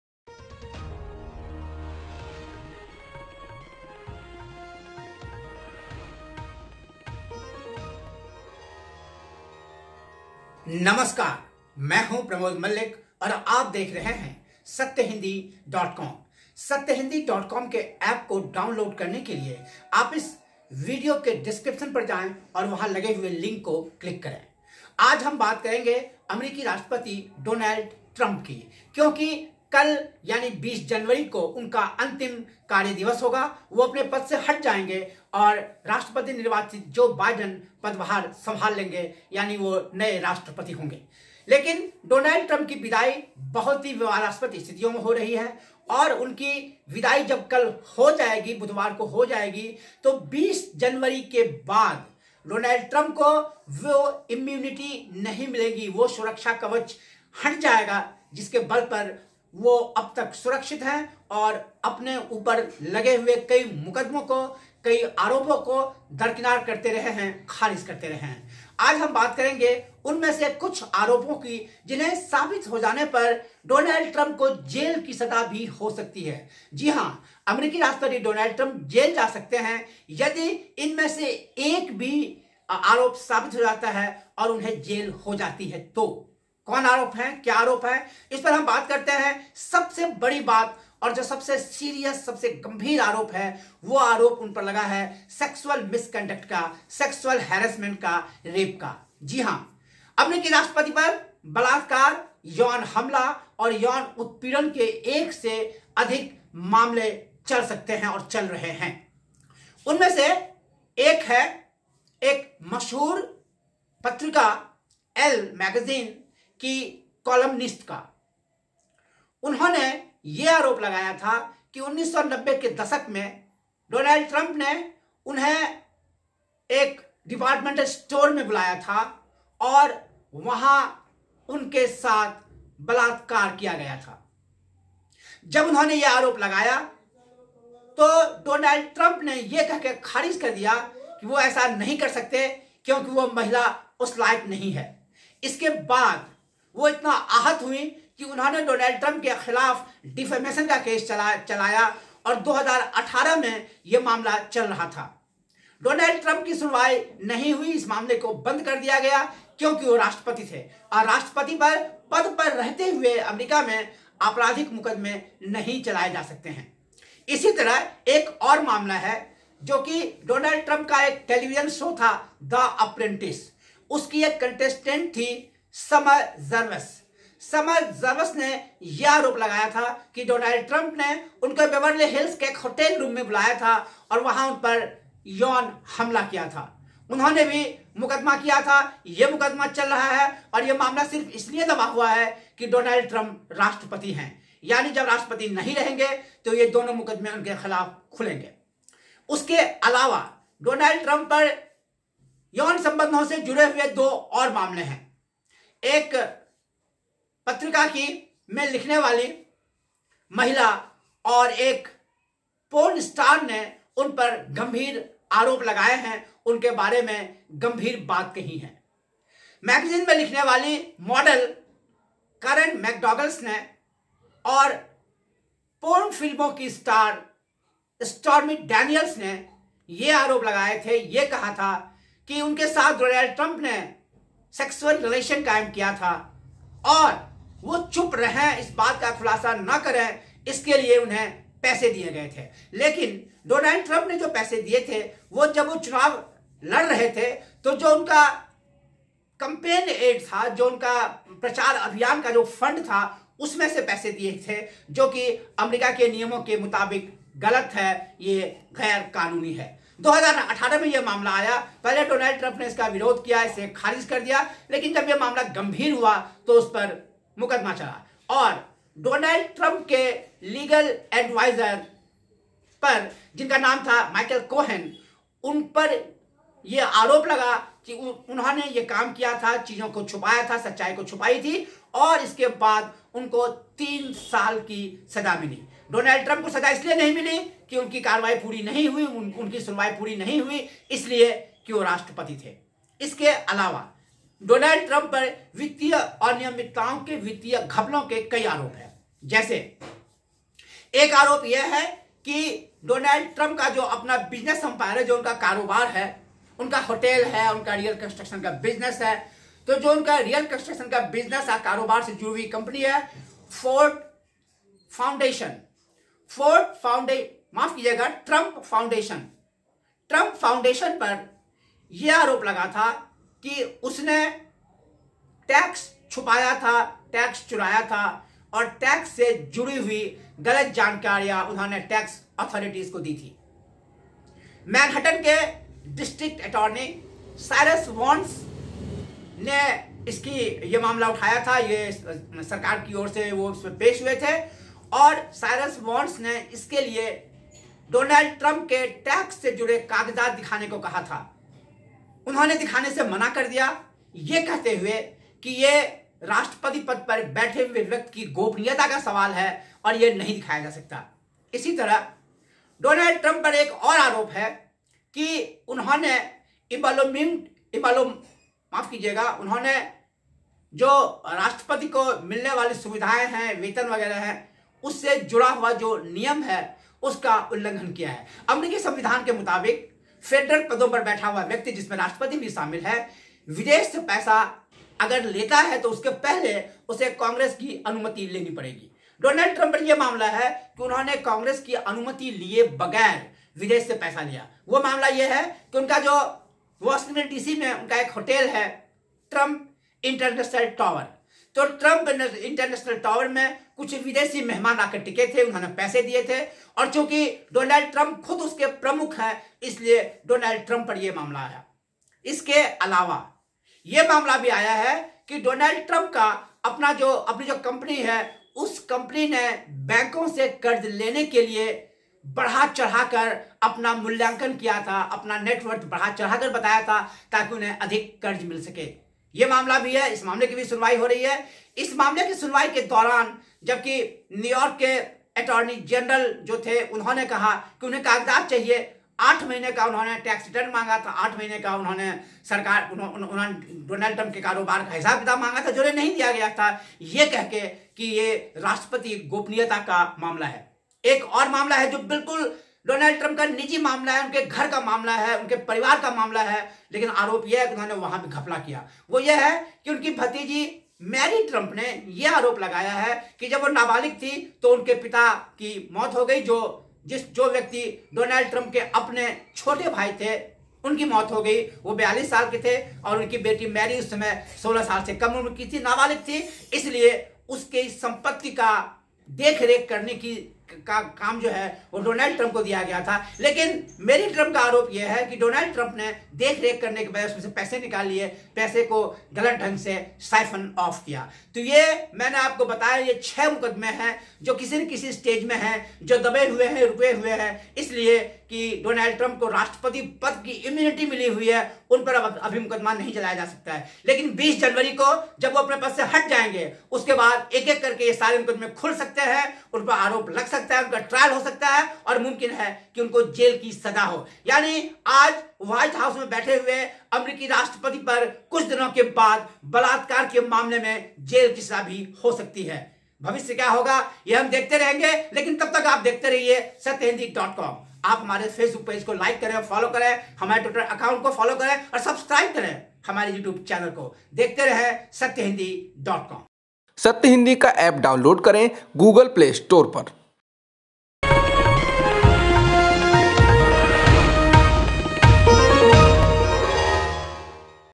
पद से हटते ही ट्रंप पर चल सकते हैं ये 6 मुक़दमे, हो सकती है जेल
राष्ट्रपति पद पर रहते हुए जिन मुक़दमों से डोनल्ड ट्रंप को छूट मिली हुई है, वे खुल सकती हैं। इनमें बलात्कार, यौन हमला, टैक्स चोरी, फ़र्जीवाड़ा और अपनी ही भतीजी का पैसा ग़लत ढंग से निकाल लेने तक के आरोप शामिल हैं। 20 जनवरी के बाद क्या उन पर ये मुक़दमे चलेंगे, क्या ट्रंप साहब जेल जाएंगे? सत्य हिन्दी पर देखें प्रमोद मल्लिक के साथ।

‘सत्य हिन्दी’ की ताक़त बनिए। हमारी सदस्यता योजना में शामिल हों